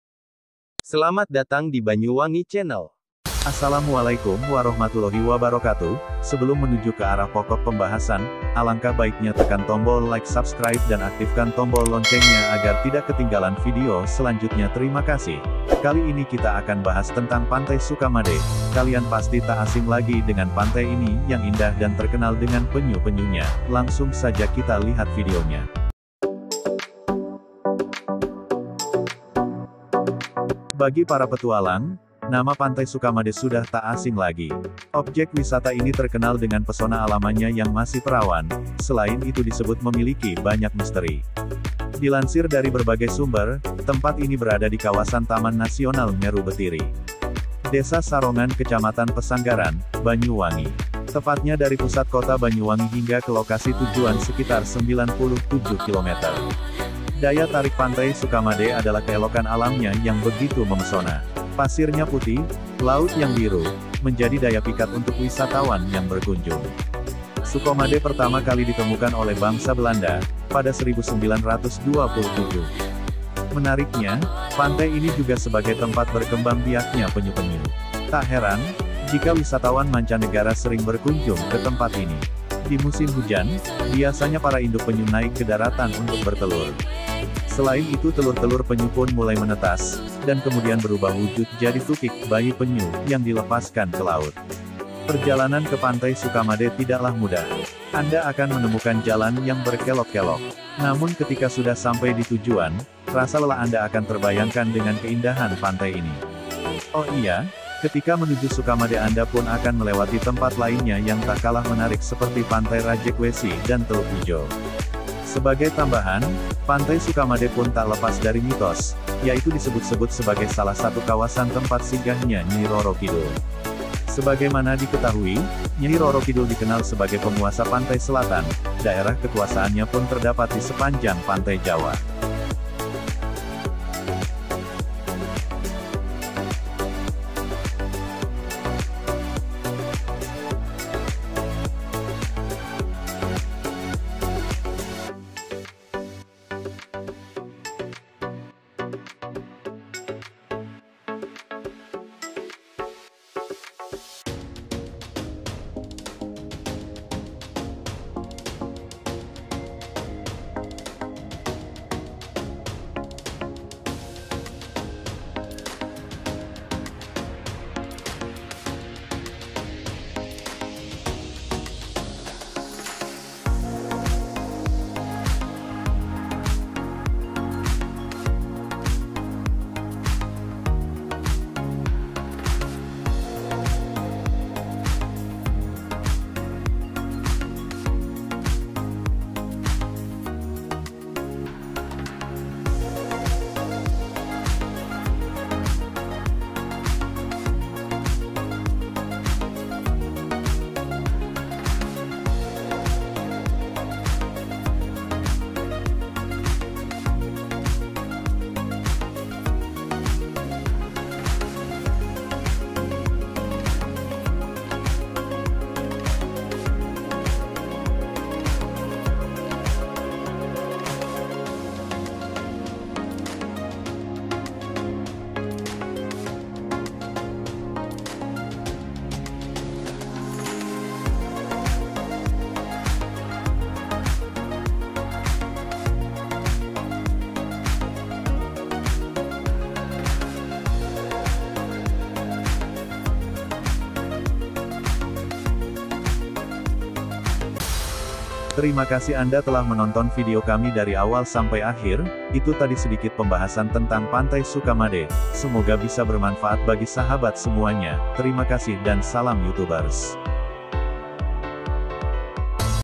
pantai sukamade banyuwangi, tempatnya habitat penyu terbesar di Indonesia | Bwi channel
Pantai Sukamade merupakan obyek wisata alam yang berada di kawasan Taman Nasional (TN) Meru Betiri, Kabupaten Banyuwangi. Keunikan Pantai Sukamade adalah pengalaman melihat penyu bertelur di malam hari yang tentunya tidak bisa didapat di sembarang pantai.